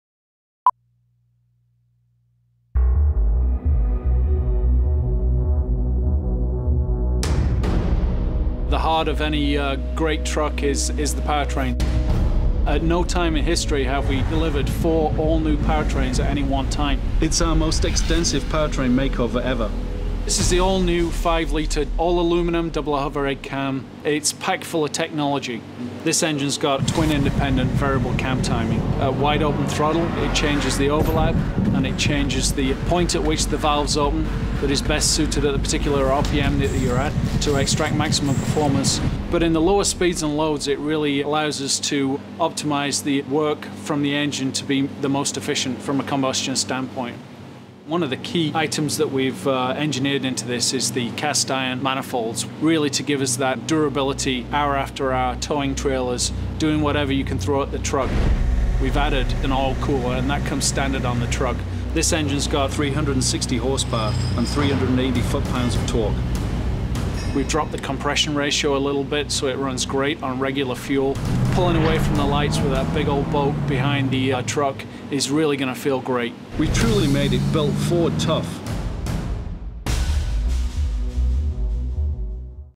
Ford F-150 New 5.0L Ti-VCT V8 Engine
The 2011 F-150 is coming with your choice of four new engines.  And everyone is a leader.  With an available 5.0L V8, also with TiVCT design, the new 5.0L engine delivers power and capability.  V8 Program Manager Mike Harrison explains why.